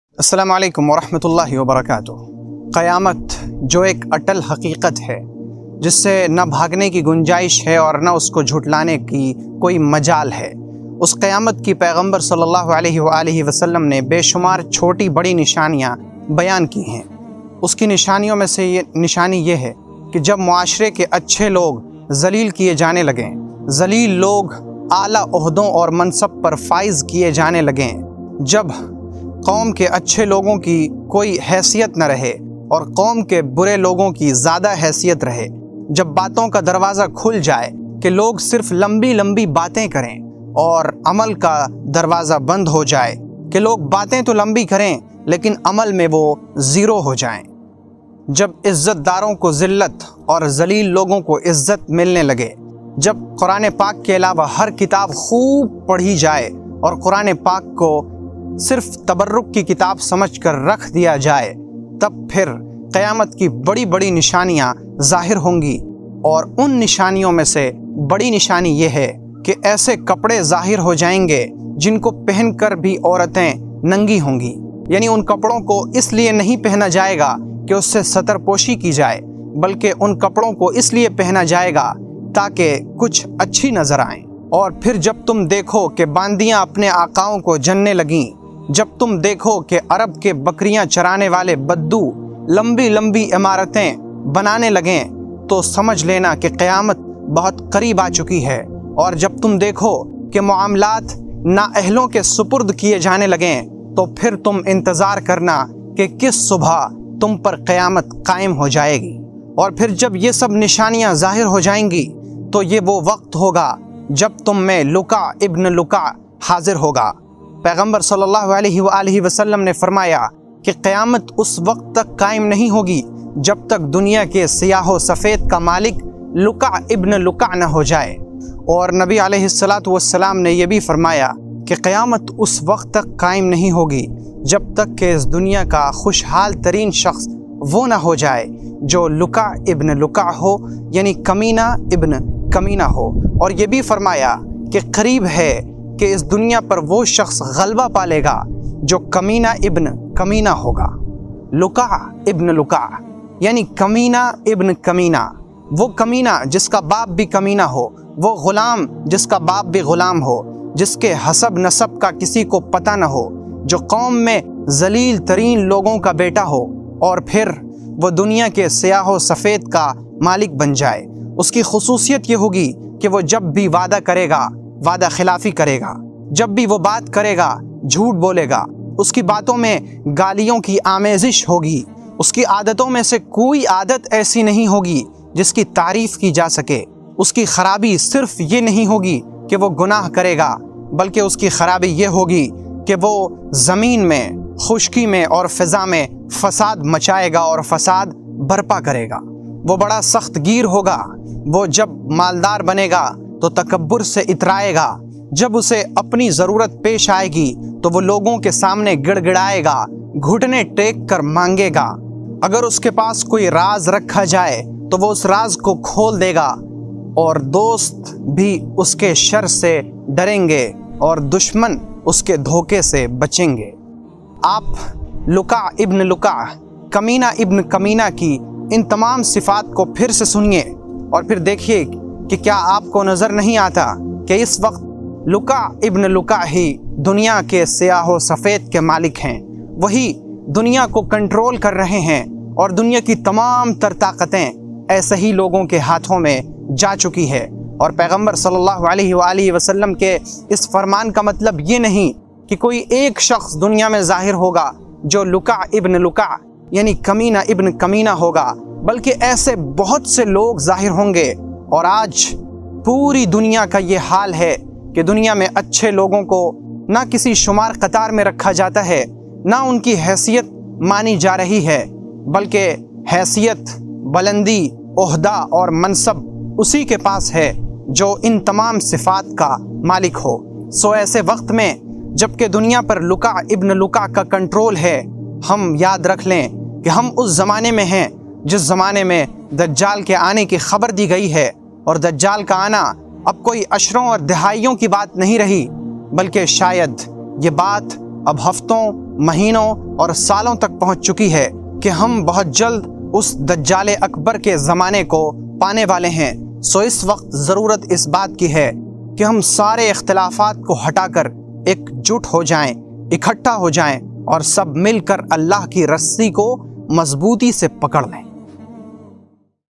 Qiyamat ki Ek badi Nishani || Yeh Aadmi Aa Gaya || Taza Fikr
Qiyamayt ki nishaniyon men se kuch khaas logon ka dunya me zaahir hona. jin men se ek Luka bin Luka hay. qiyamat ki yeh nishani puri ho chuki, aur yeh luka bin luka zaahir ho chuka hay.
dajjal bhi Qiyamayt ki badi nishanion men se hain, dunya men bahut jald woh zaahir hone wala hay. Allah hamen uske fitne se bachaye rakhey.